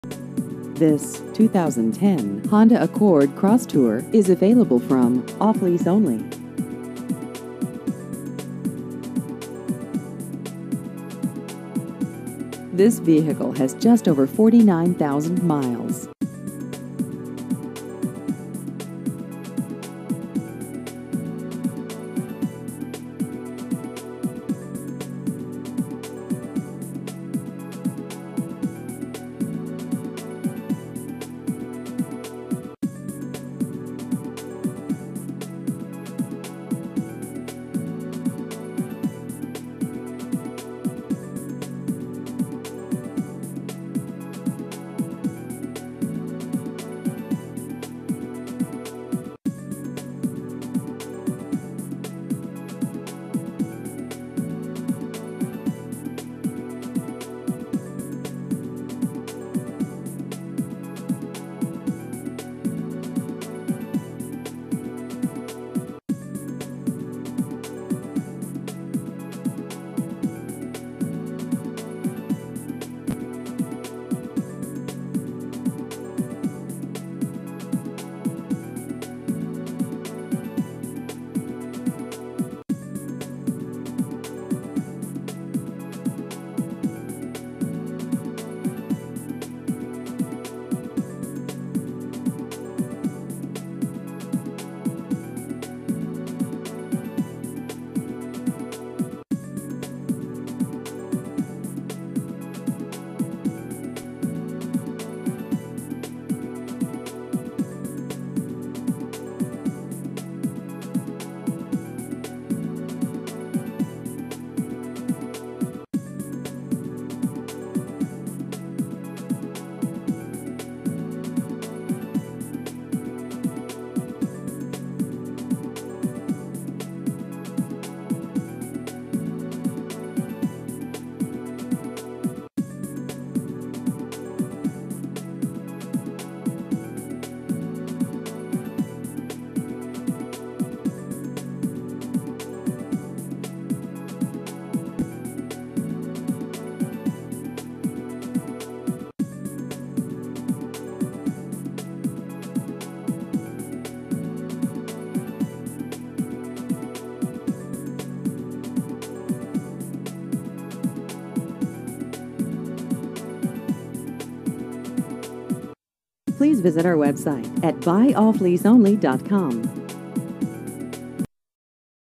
2010 Honda Accord Crosstour Used Cars Lake Worth FL
This 2010 Honda Accord Crosstour is available from Off Lease Only.